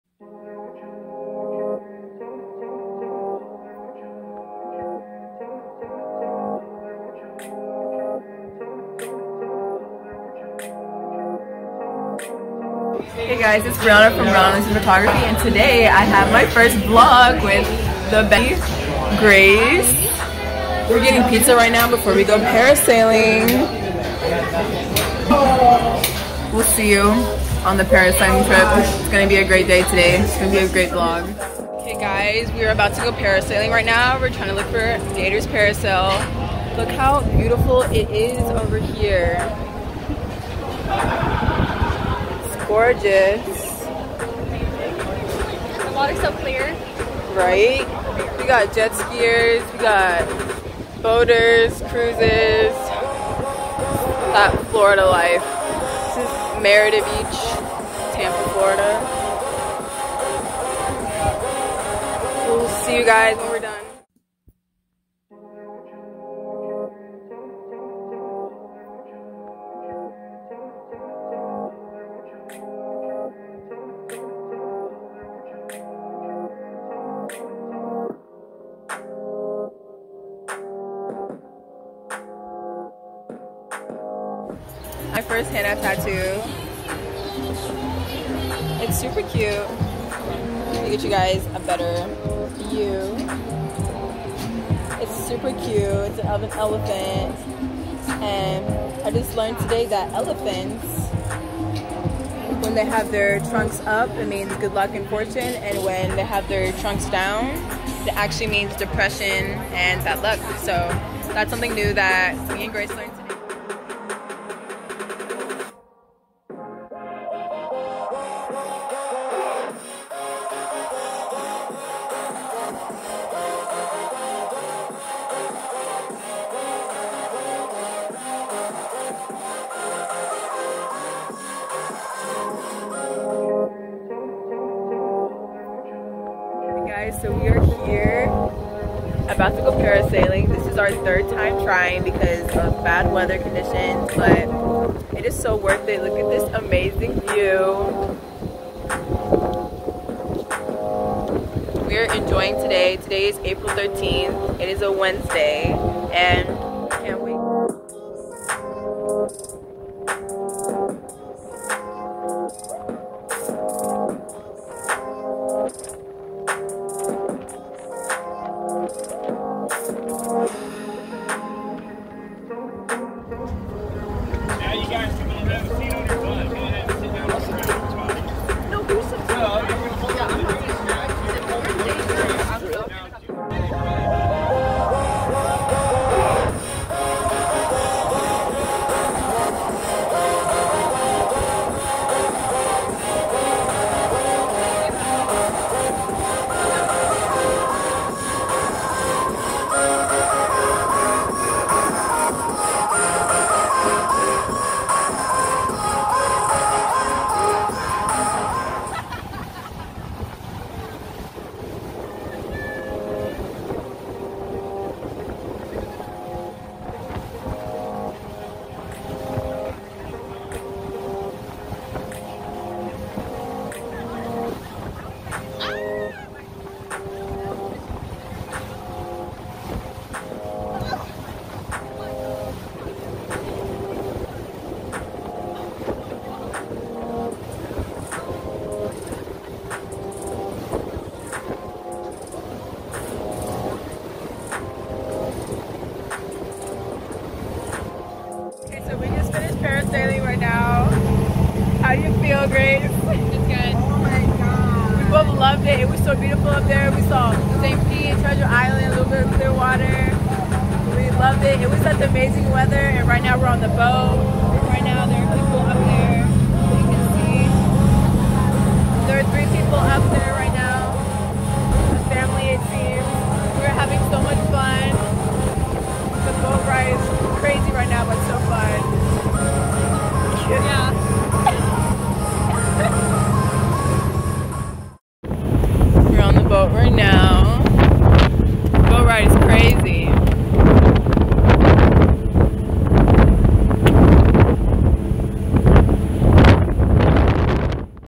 PARASAILING VLOG | Spring Break 2022 Ft. Gators Parasail (Madeira Beach Florida)
My first time parasailing for spring break on April 13th 2022 at Gator parasail in Madeira beach! Everything went well and I would recommend this company when you visit. I apologize for the annoying watermarks! This is my first time using this editor and I didn't know it would have that.